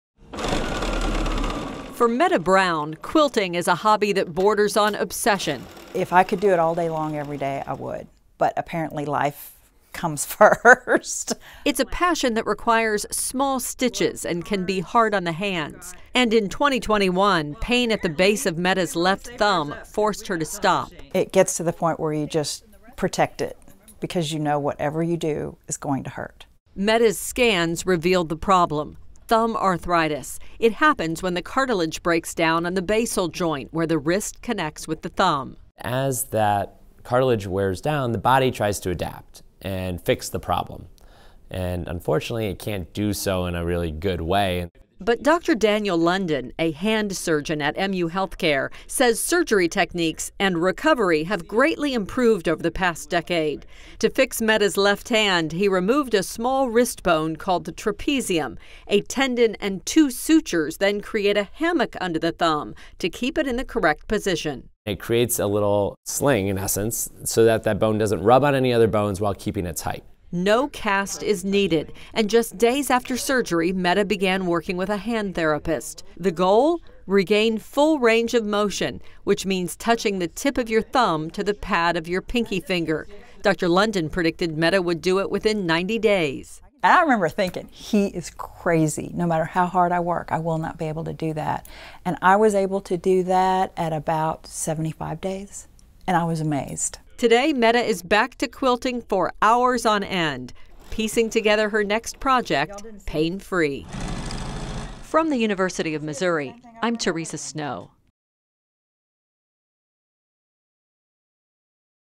Thumb Surgery Offers Arthritis Relief, Quick Recovery (Daniel London, MD)
Many older adults, particularly women over age 50, endure pain at the base of their thumbs and lose grip strength and range of motion. These are symptoms of thumb arthritis, a common condition that makes it hard to twist the cap off a bottle, use a doorknob or button a shirt.